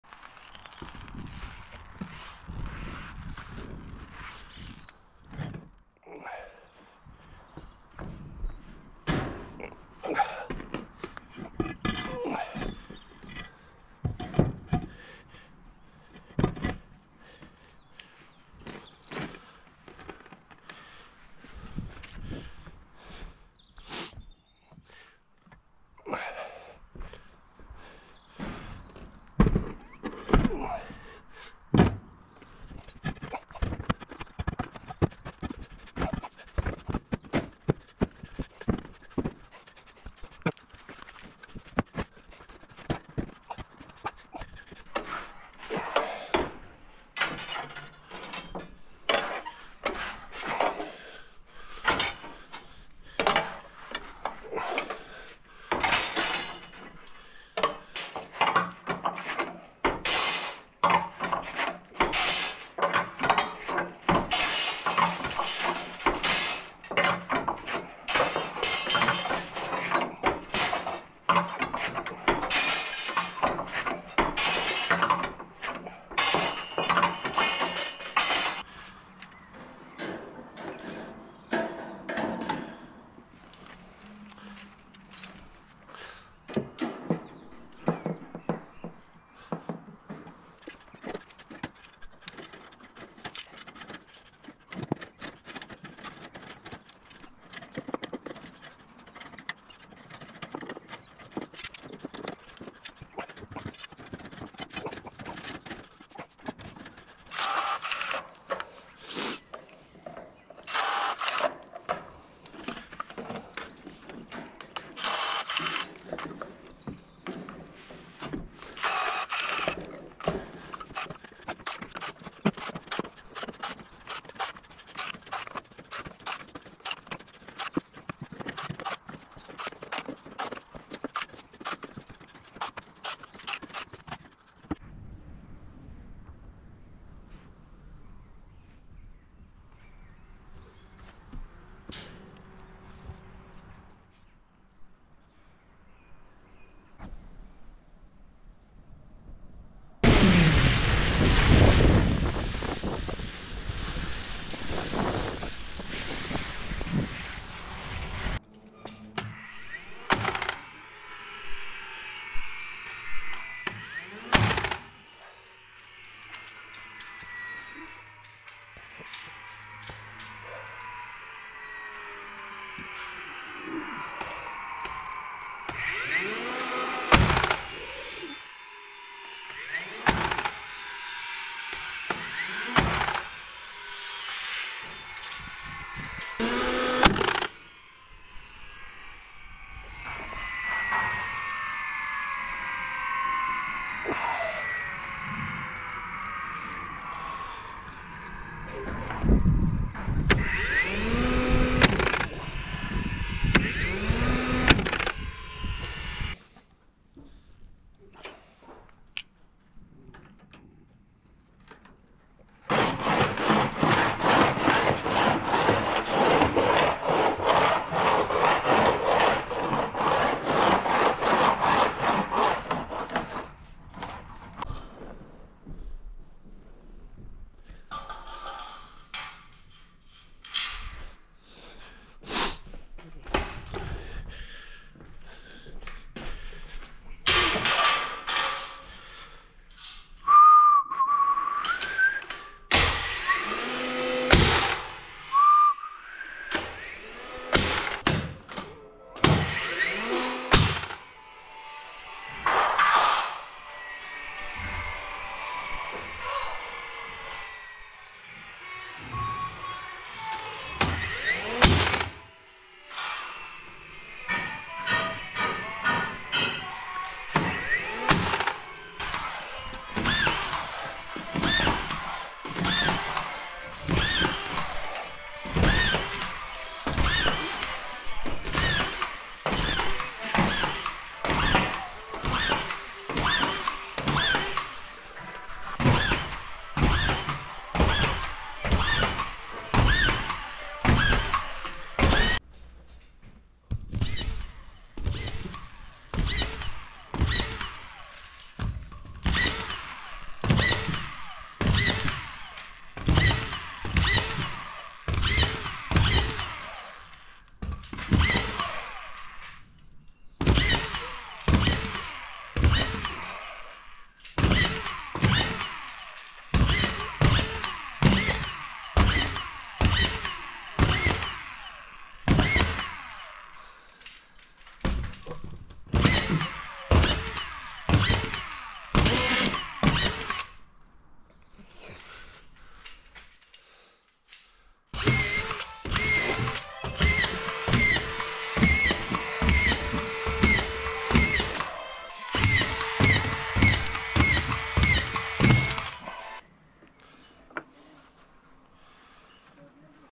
3. Ferraillage dalle hourdi, Construction de maison de A a Z
Dans vidéo je crée l'armature, je lie les renforts d'ange et je coule le béton de la dalle du premier étage.